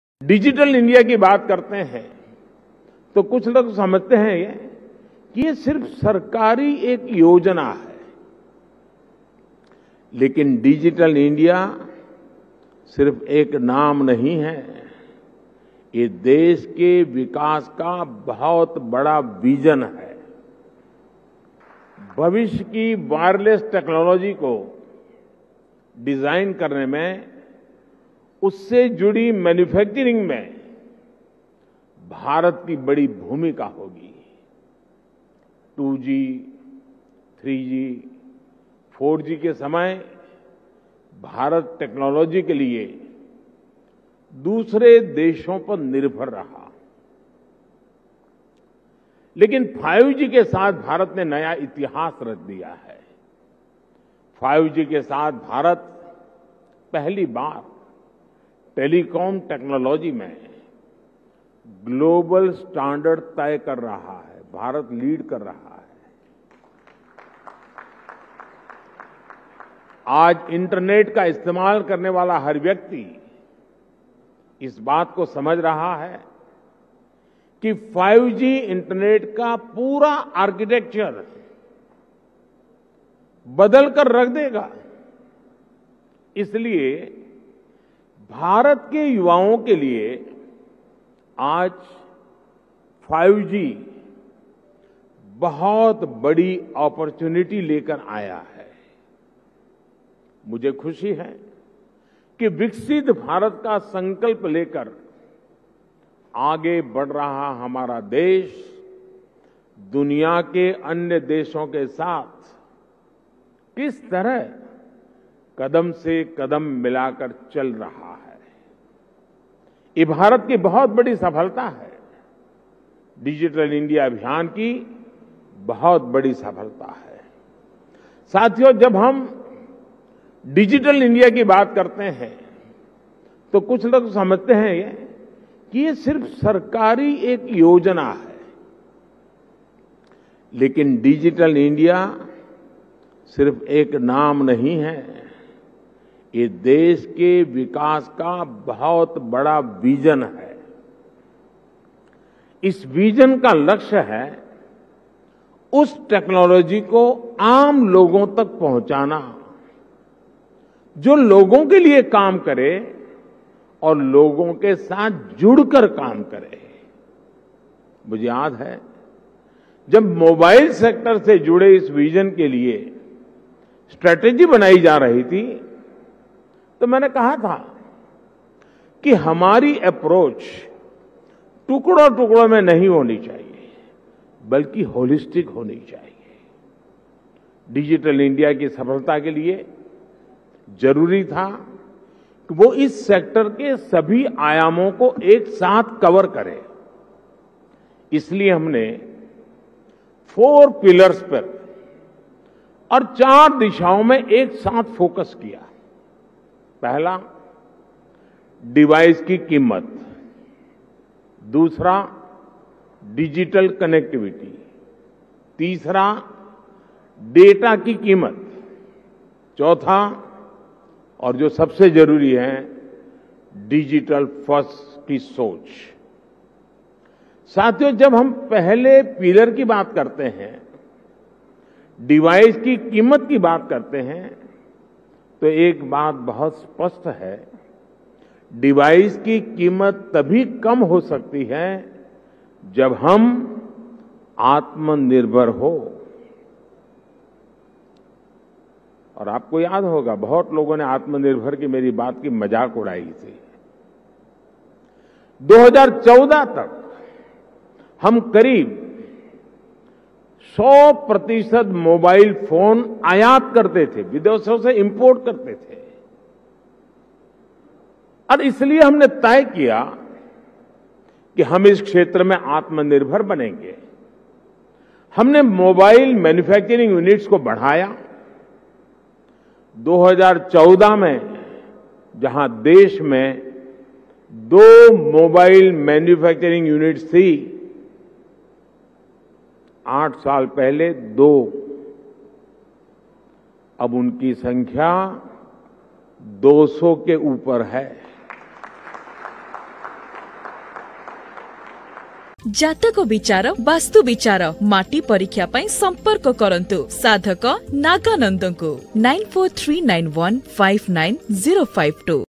5G ସହିତ ଭାରତ ନୂଆ ଇତିହାସ ରଚିଛି । ଡିଜିଟାଲ୍ ଇଣ୍ଡିଆ, ଭାରତ ପାଇଁ ଏକ ନୂଆ ଦିଗ ଦେଇଛି
5G ସହିତ ଭାରତ ନୂଆ ଇତିହାସ ରଚିଛି । ଡିଜିଟାଲ୍ ଇଣ୍ଡିଆ, ଭାରତ ପାଇଁ ଏକ ନୂଆ ଦିଗ ଦେଇଛି ।